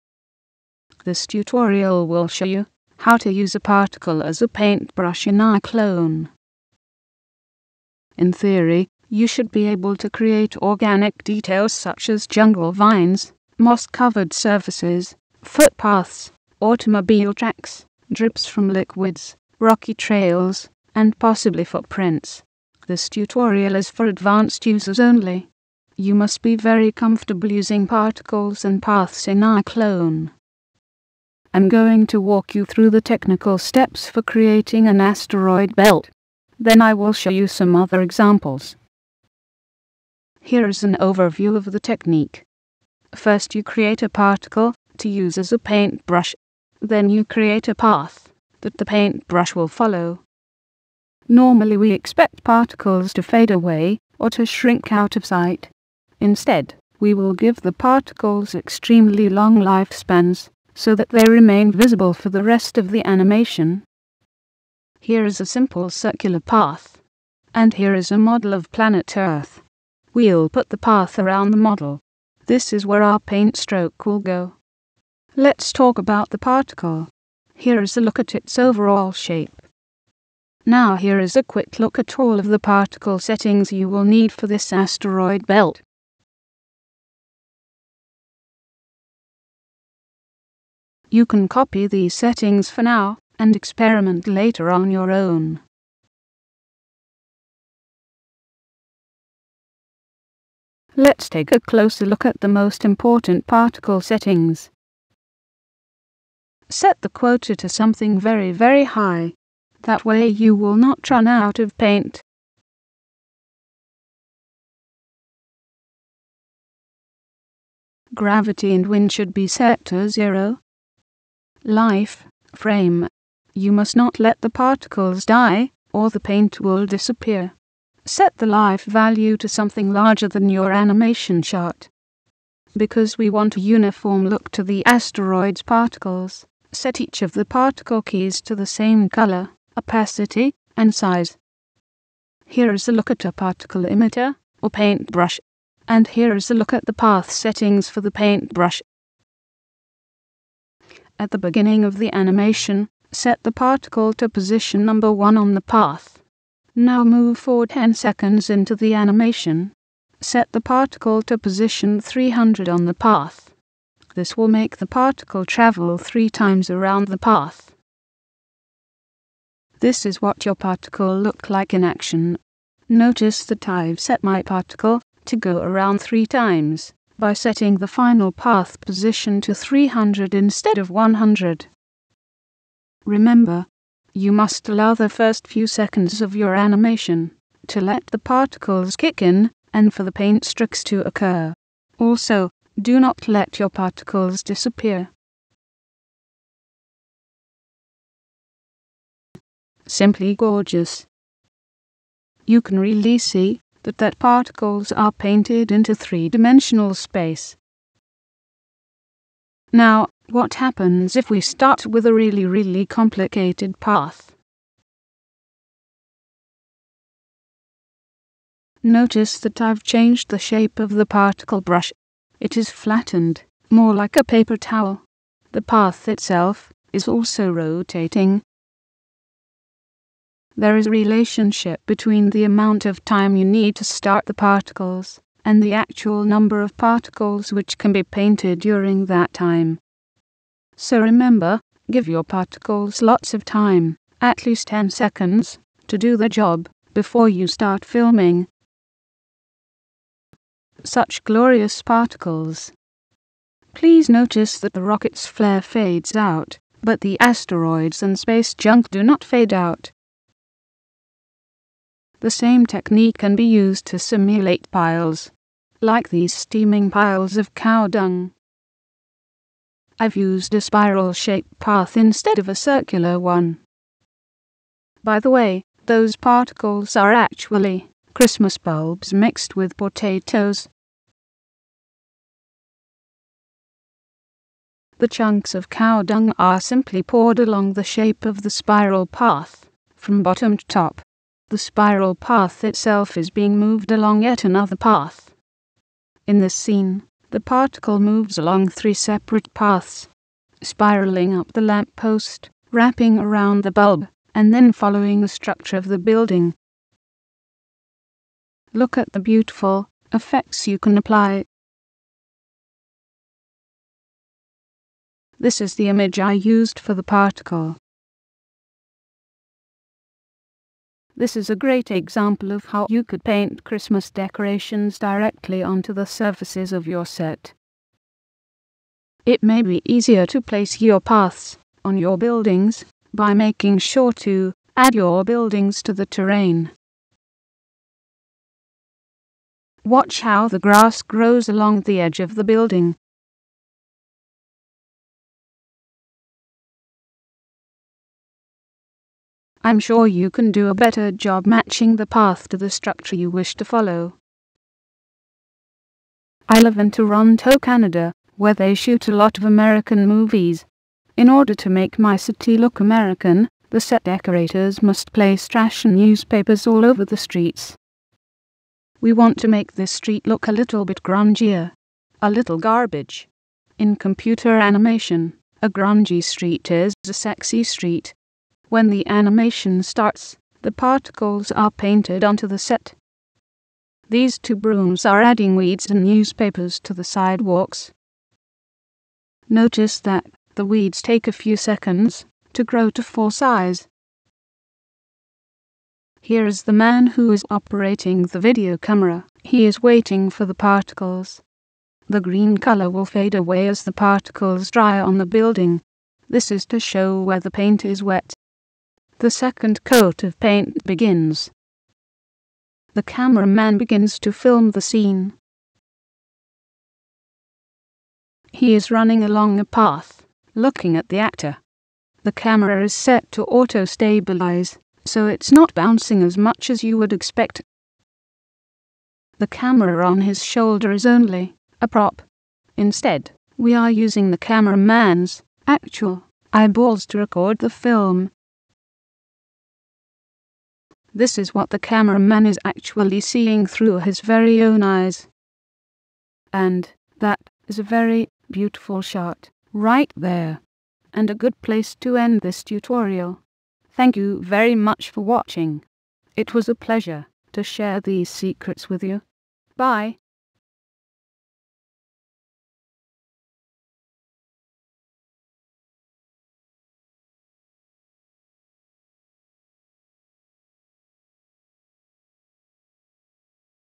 iClone 4 Tutorial: Using Particles as Paintbrushes
JasonJ developed a technique for painting effects directly onto scenery in iClone, called "Dirt Painting".  With this technique you can paint ivy, dirt, old newspapers onto buildings and edges..